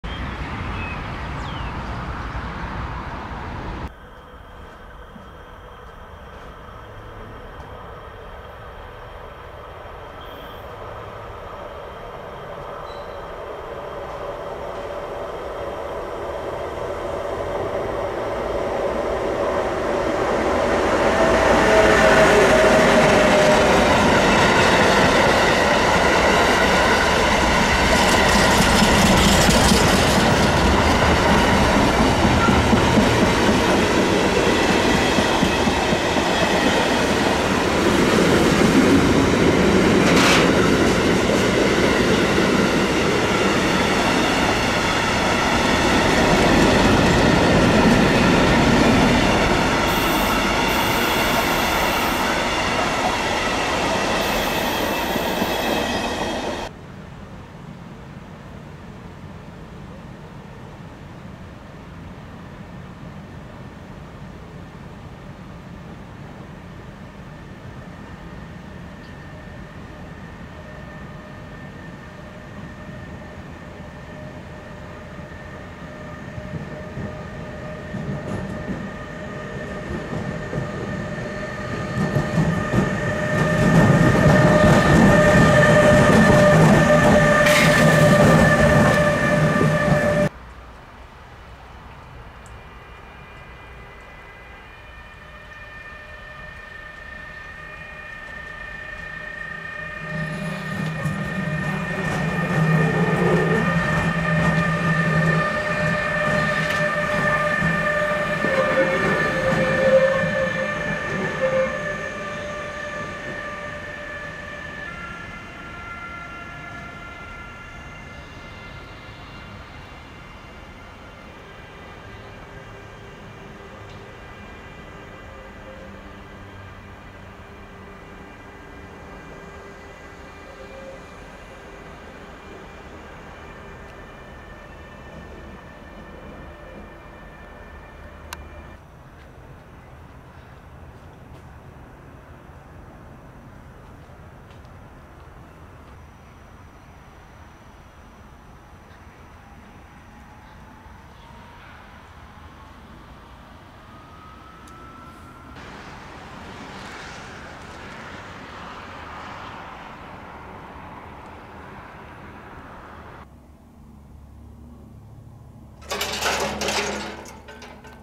🟧🟨🟩🟦ÖBB Terminal Wolfurt: LINEAS❗ jetzt❗durchgängig am Antwerpen-Shuttle am Containerzug mit BR186
Seit Dezember 2021 kommt der Antwerpen-Shuttle direkt mit LINEAS, zuvor haben RTS bzw. RHC den Containerzug ab Augsburg gebracht. Am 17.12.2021 konnte ich den bei der Ankunft am Terminal Wolfurt sehen.